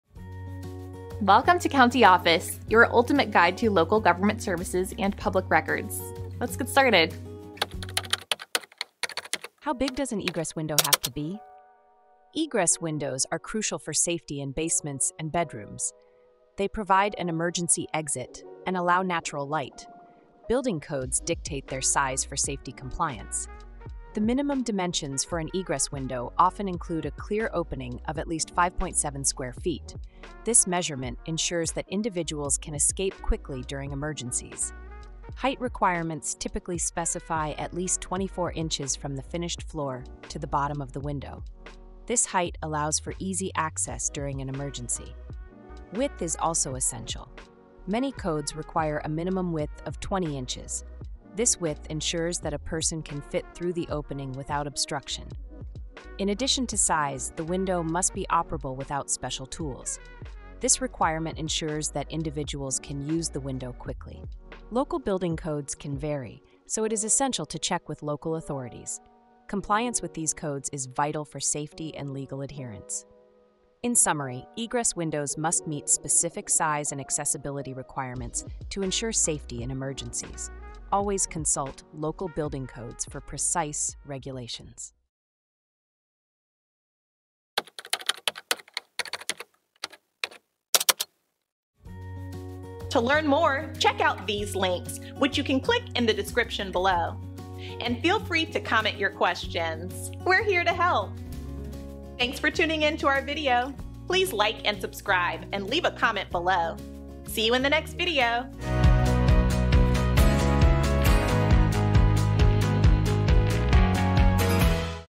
How Big Does An Egress Window Have To Be? - CountyOffice.org
How Big Does An Egress Window Have To Be? In this educational video, we delve into the essential topic of egress windows and their critical role in ensuring safety in basements and bedrooms. Building codes mandate specific size requirements for egress windows to guarantee compliance with safety standards. The minimum dimensions, including a clear opening of at least 5.7 square feet, are crucial for facilitating quick escapes during emergencies. Height and width requirements are also specified to ensure easy access without obstruction.

Moreover, the operability of the window without special tools is a key factor in emergency situations. It is imperative to adhere to local building codes, as they may vary, emphasizing the importance of safety and legal compliance. To learn more about the specific regulations surrounding egress windows and their impact on safety, watch this insightful video and stay informed.